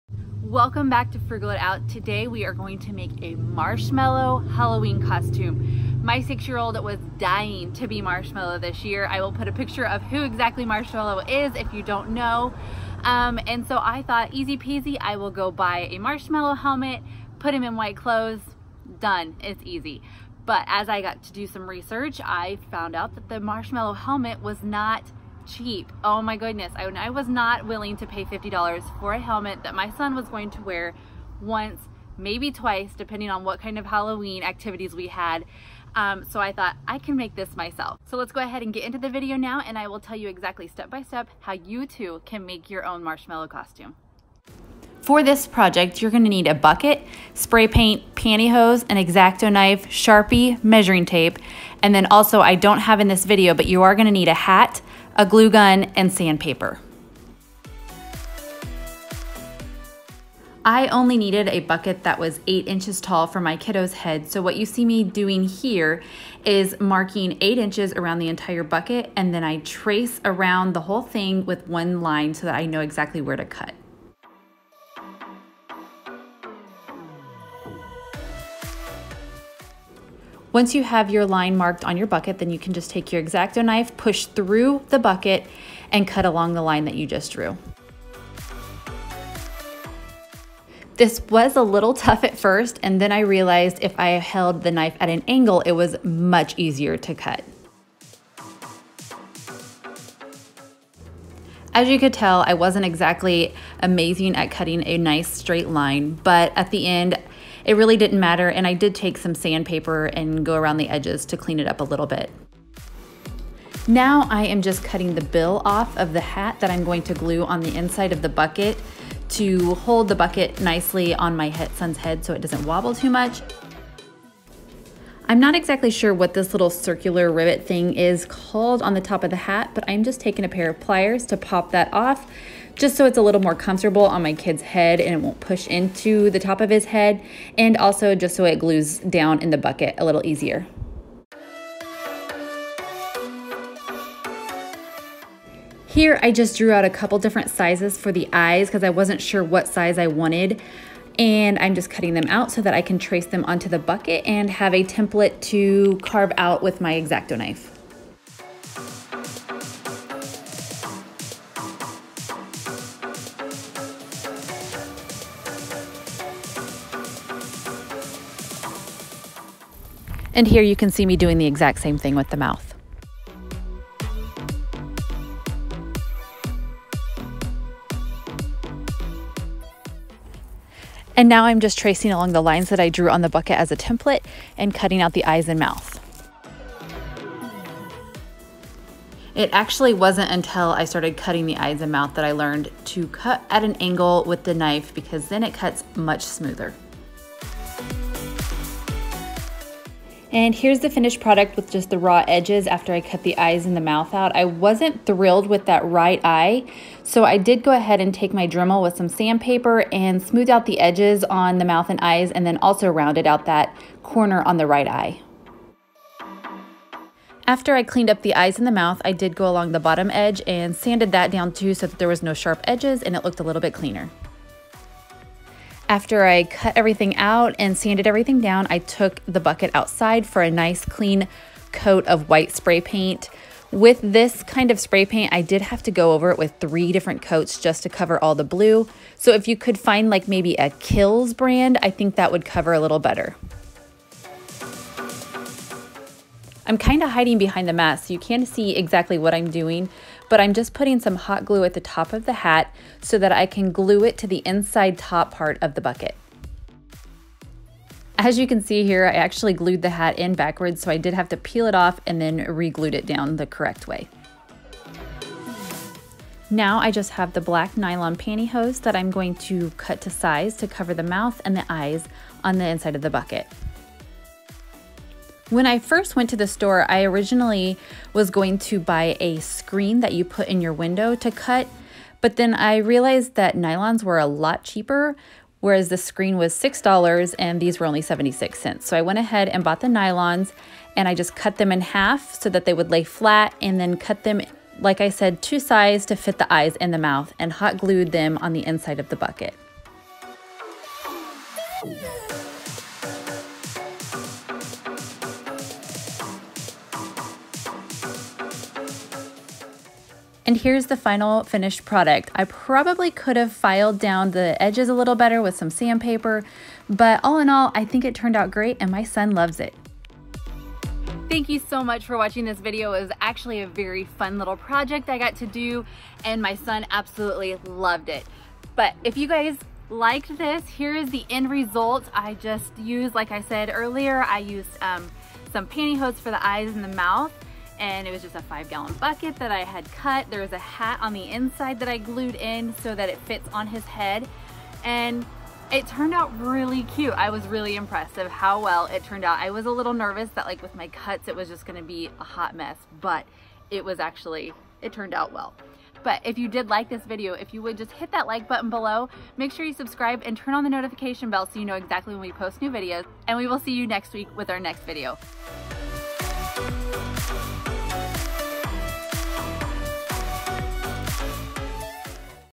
DIY Marshmello Costume
My son wanted to be Marshmello the DJ for Halloween but I didn’t want to fork out a lot of money on a costume he will only wear once so I figured out a way to make it myself!

Spray Paint
Hot Glue Gun
Box Cutter
Black Knee-high Panty Hose
Old Hat
Sand Paper
Sharpie
Measuring Tape